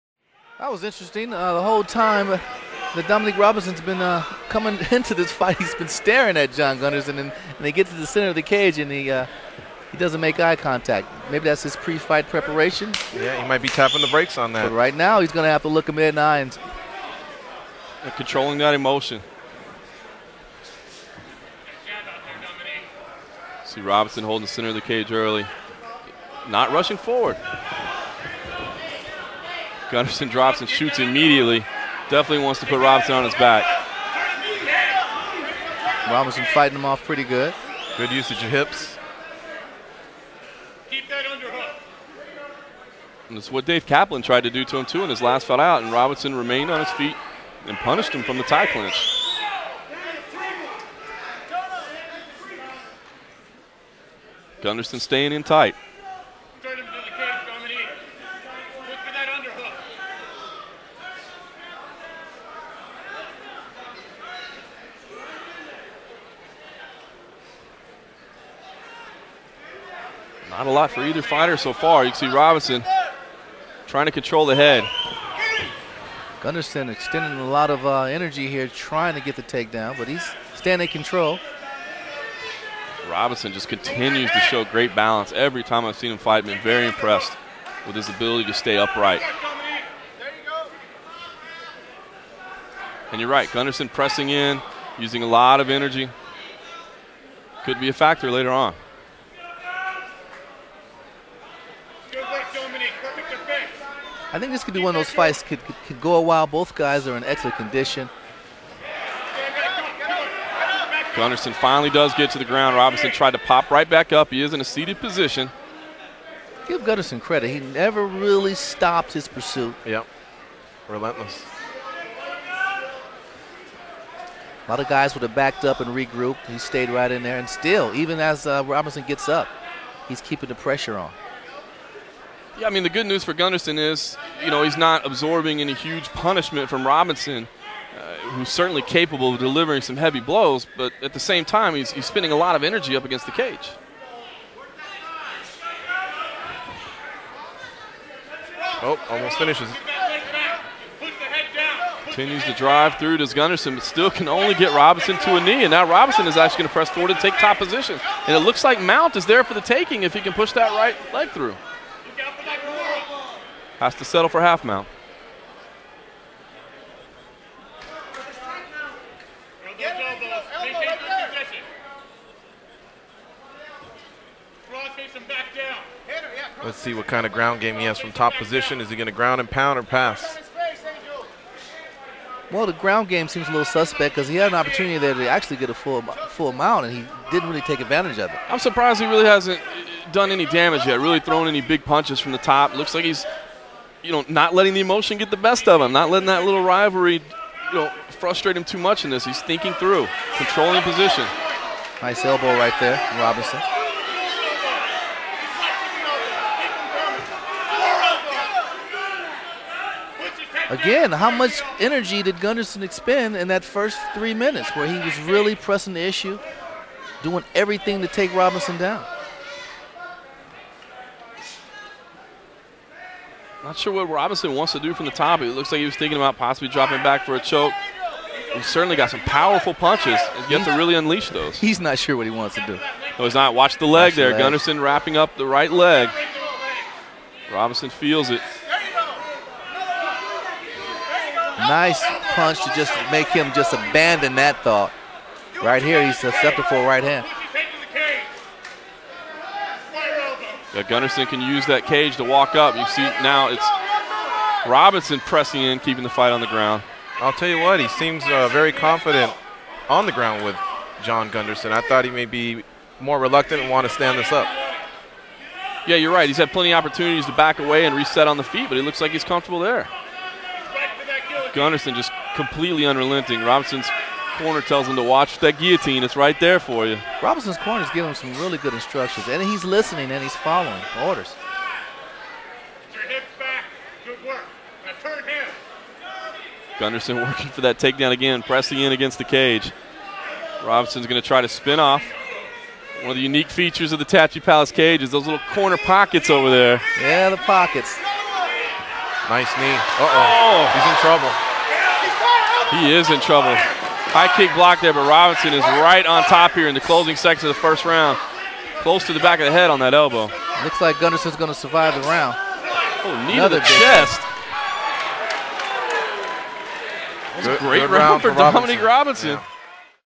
"Fallen Angel" Dominique Robinson vs John Gunderson Rnd 1
TPF 8: All or Nothing Feb 2011. "Fallen Angel" Dominique Robinson vs John "Quick Guns" Gunderson ROUND 1